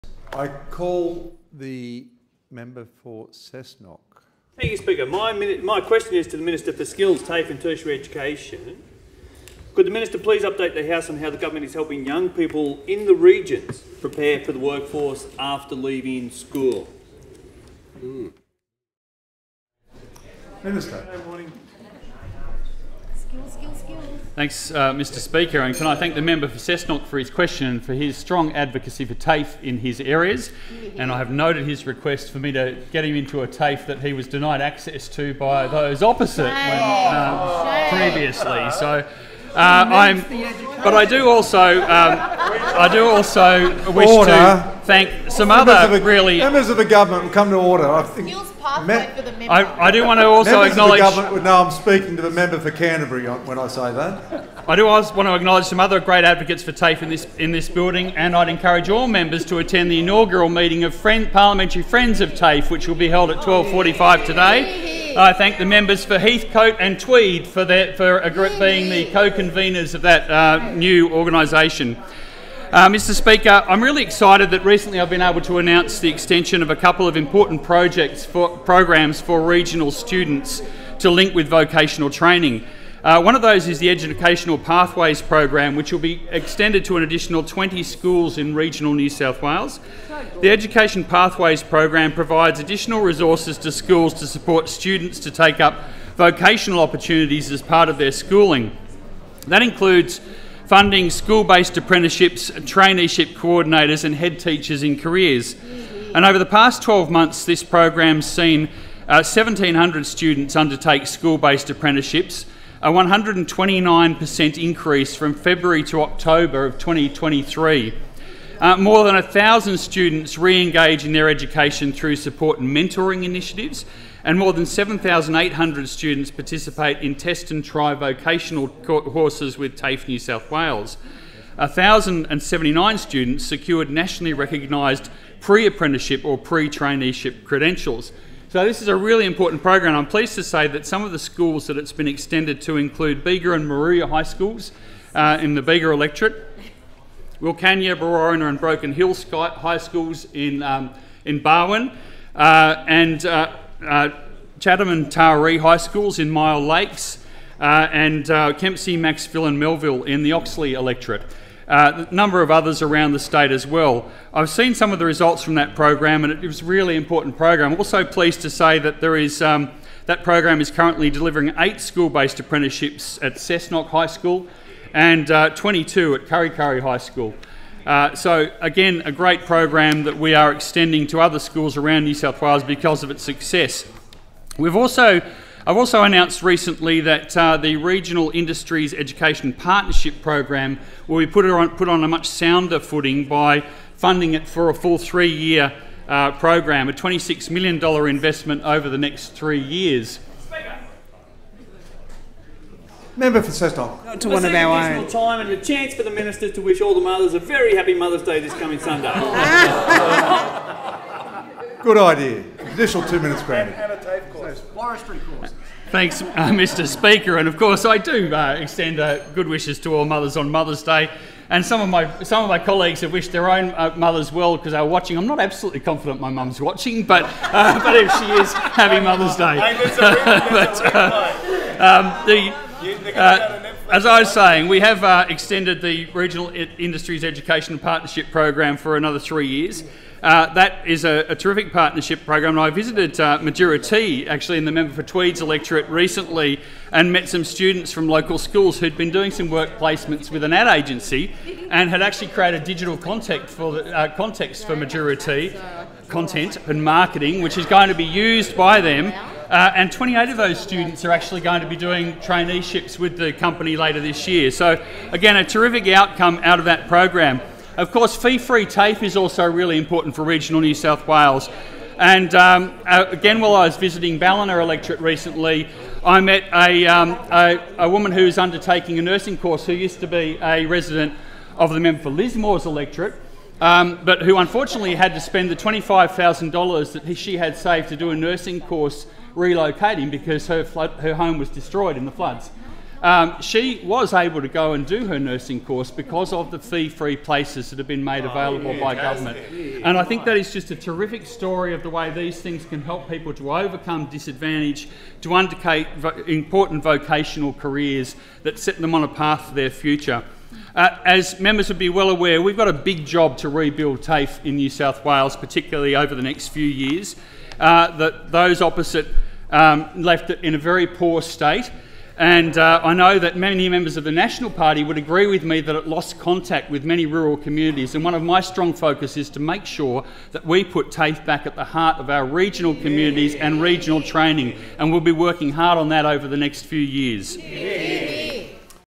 We're rebuilding TAFE and vocational training in the regions
I was asked by the member for Cessnock what the NSW Government is doing to prepare our regional school leavers for the workforce and further education.